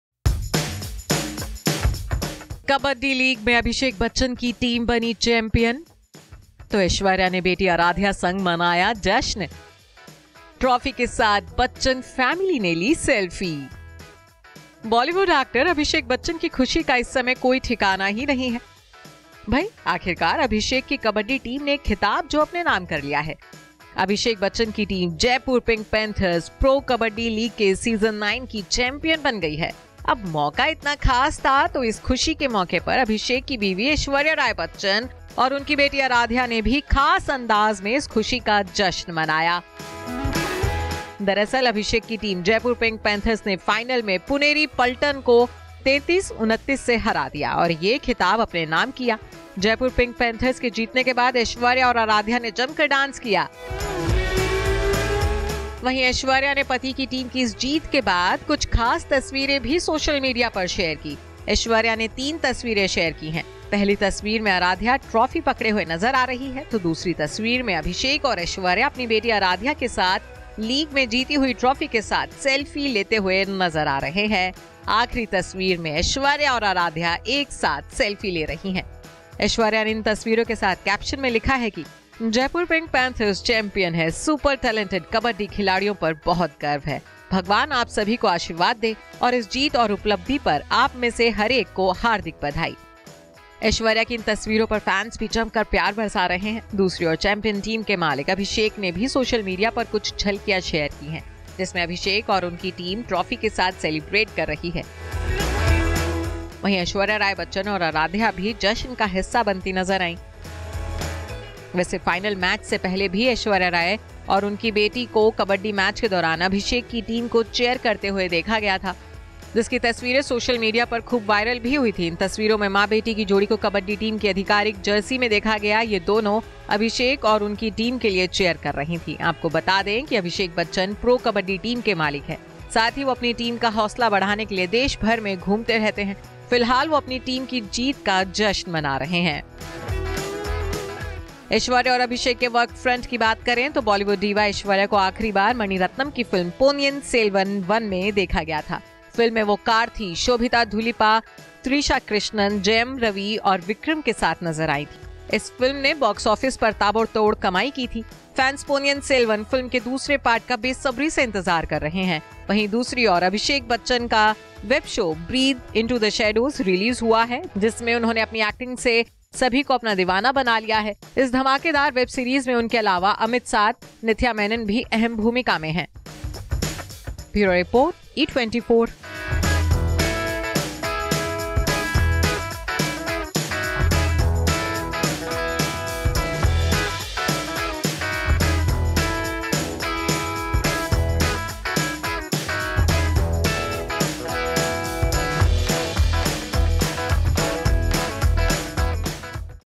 Abhishek Bachchan की टीम ने कबड्डी लीग में लहराया जीत का परचम, Aishwarya-Aaradhya ने किया सेलिब्रेट
बॉलीवुड एक्टर अभिषेक बच्चन की ख़ुशी का इस समय कोई ठिकाना ही नहीं है...भई...आखिर अभिषेक की कबड्डी टीम ने एक ख़िताब जो अपने नाम कर लिया है..अभिषेक की टीम जयपुर पिंक पैंथर्स ने ‘प्रो कबड्डी लीग-9’ की चैंपियन बन गई है...अब मौका इतना खास था तो इस खुशी के मौके पर अभिषेक की बीवी ऐश्वर्या राय बच्चन और उनकी बेटी आराध्या ने भी खास अंदाज़ में इस ख़ुशी का जश्न मनाया... अभिषेक की टीम ‘जयपुर पिंक पैंथर्स’ ने फाइनल में पुनेरी पलटन को 33-29 से हरा दिया और ये खिलाब अपने नाम किया।